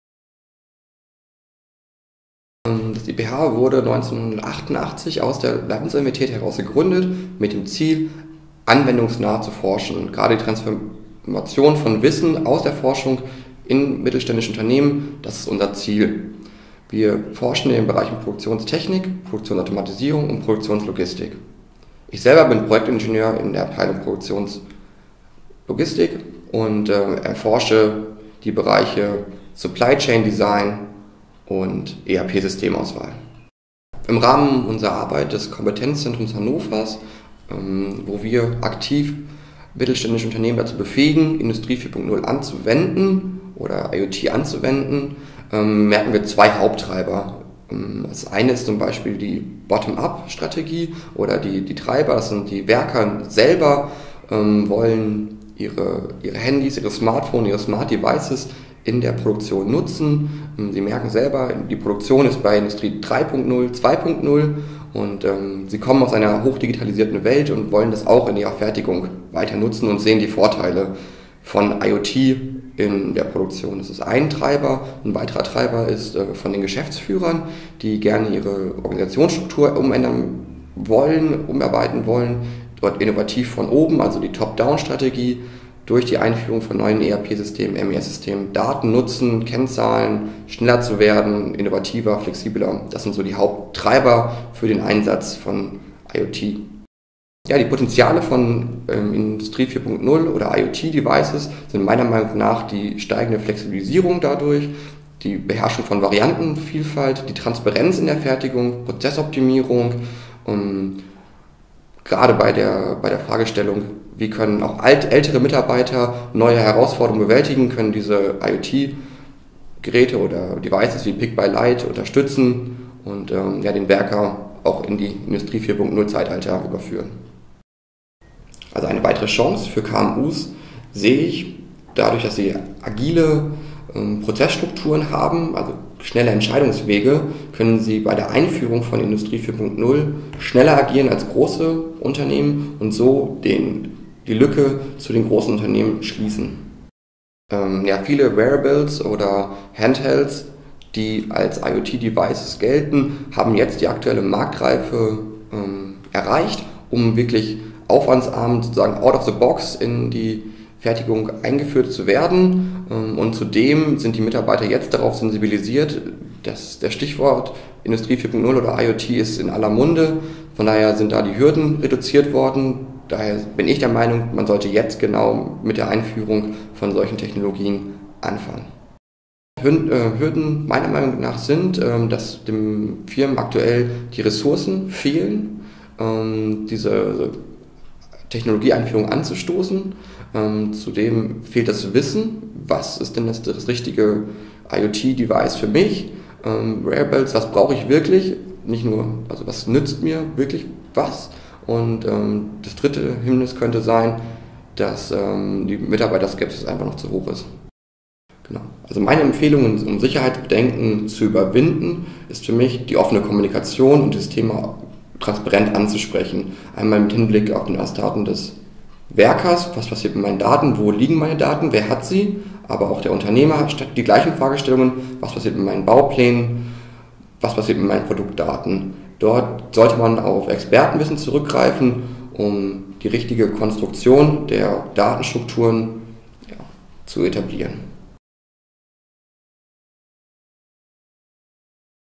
Martin Westbomke, Project Engineer at IPH gGmbH
Martin Westbomke describes the IPH's field of work as well as why the time is right for SMEs to jump into IoT.